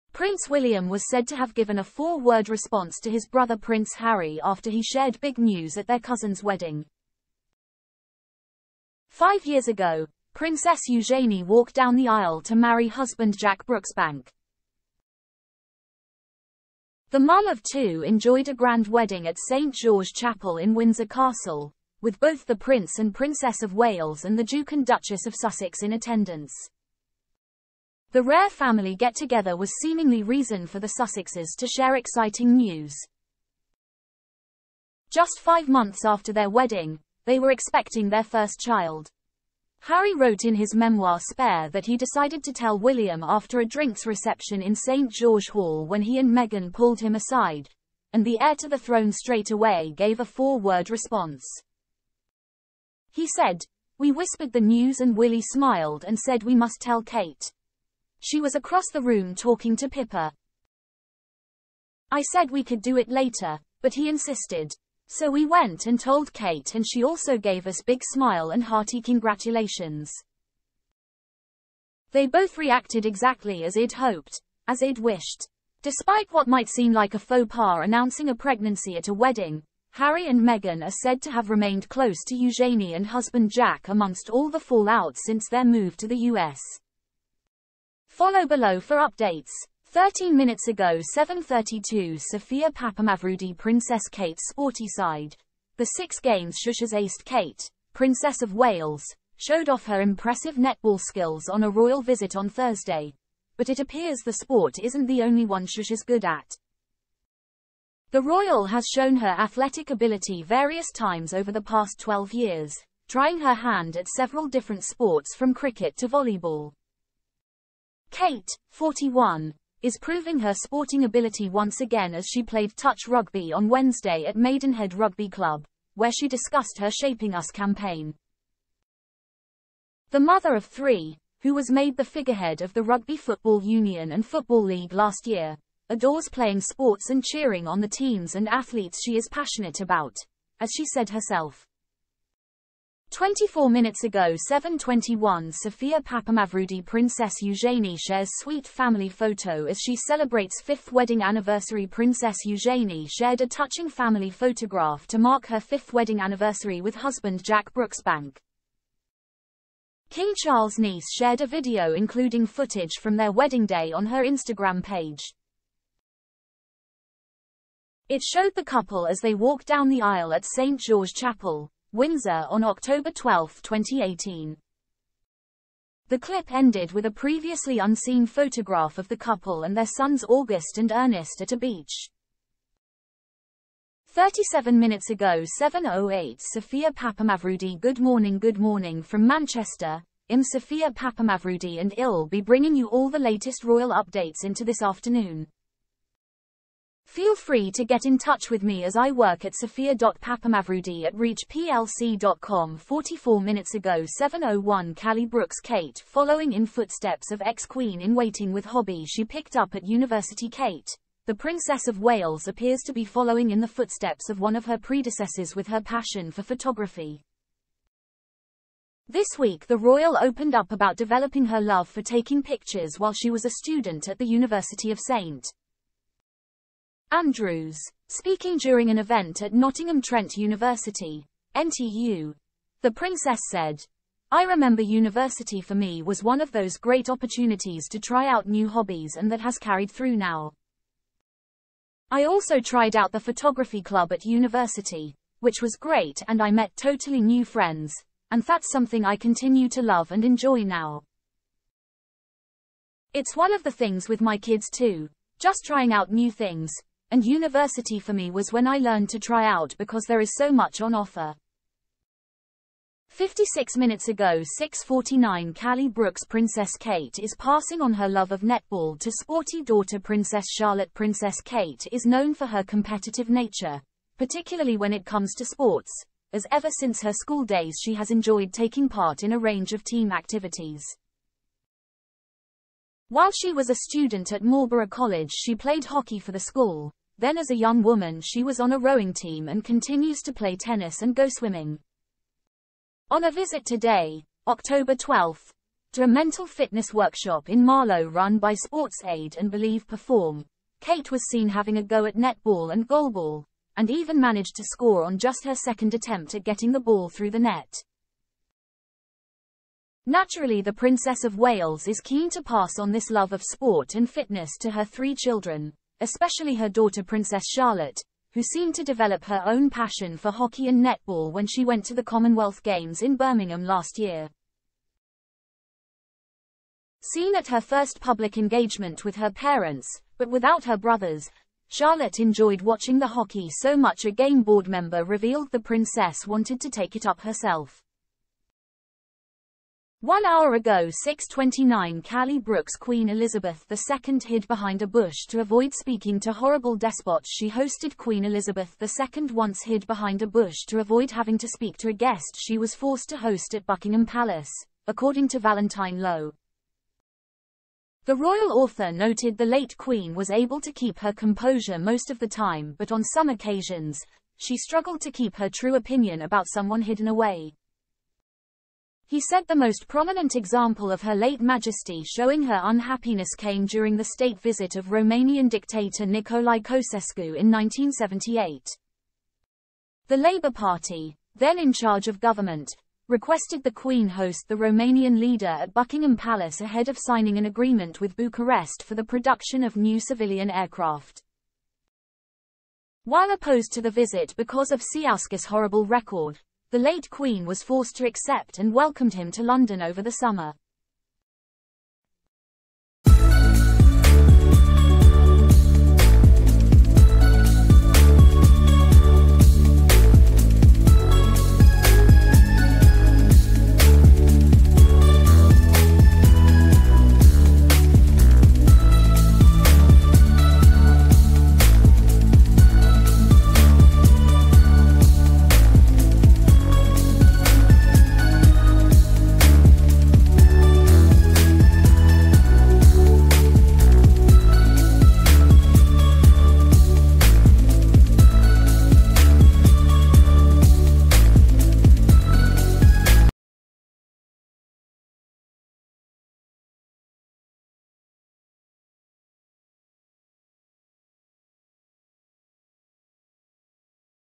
Prince Harry’s ‘faux pas’ at wedding and William’s four-word response
Prince William was said to have given a four word response to his brother Prince Harry after he shared big news at their cousin's wedding. Five years ago, Princess Eugenie walked down the aisle to marry husband Jack Brooksbank. The mum-of-two enjoyed a grand wedding at St George's Chapel in Windsor Castle, with both the Prince and Princess of Wales and the Duke and Duchess of Sussex in attendance. The rare family get together was seemingly reason for the Sussexes to share exciting news. Just fi...